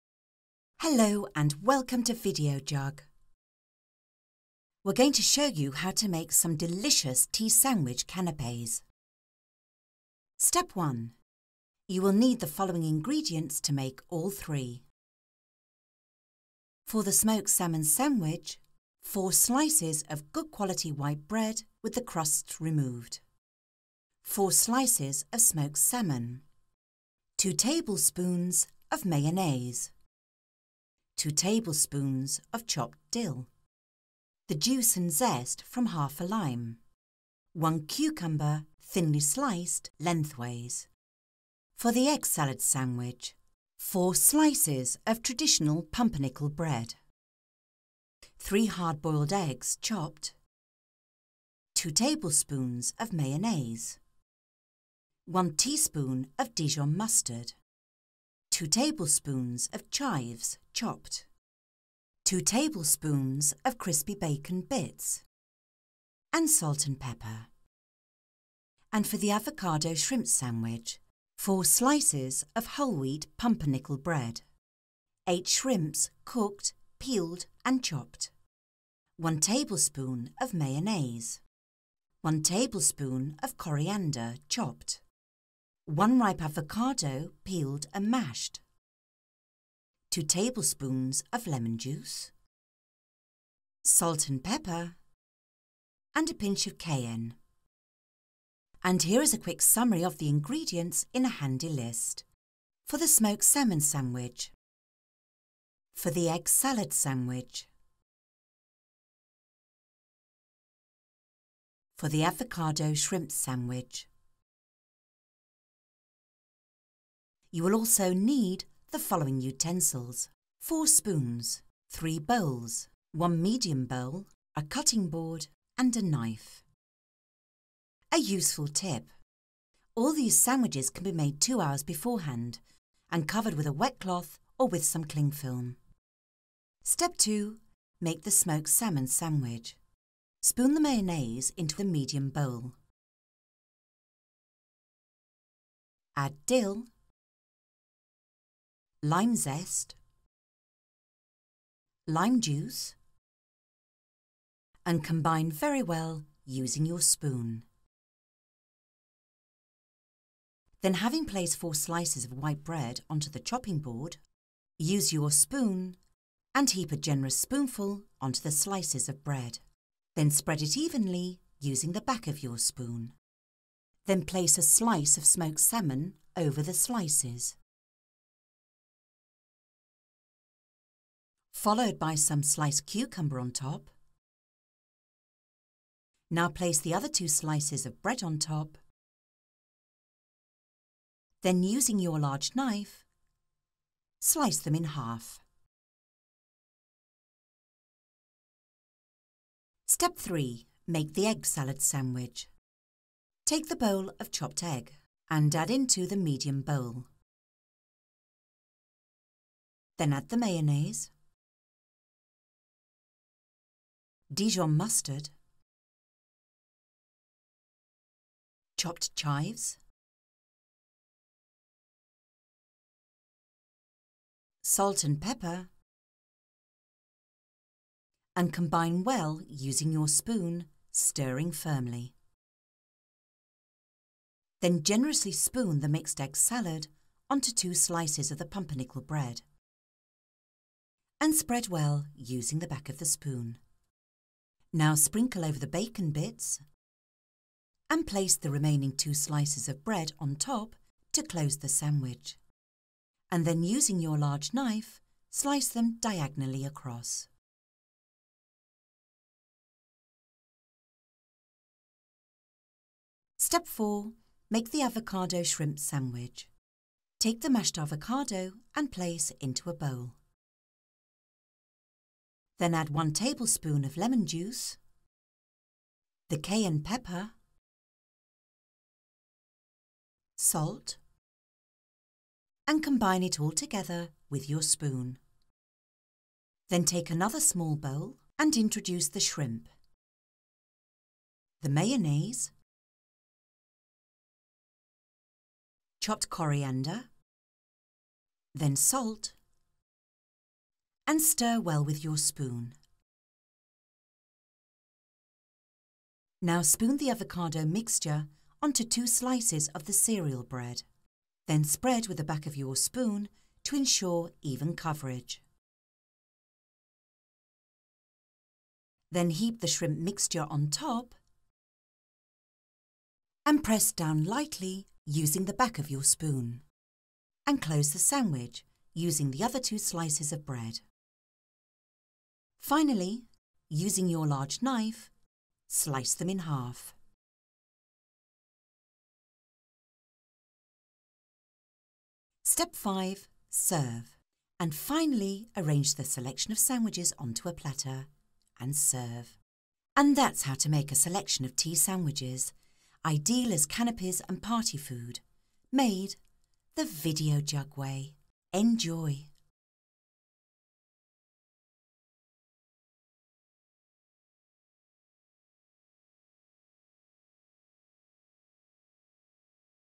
How To Make Tea Sandwiches
We show you how to make tea sandwiches.

Are you brave enough to try this mac and cheese pizza?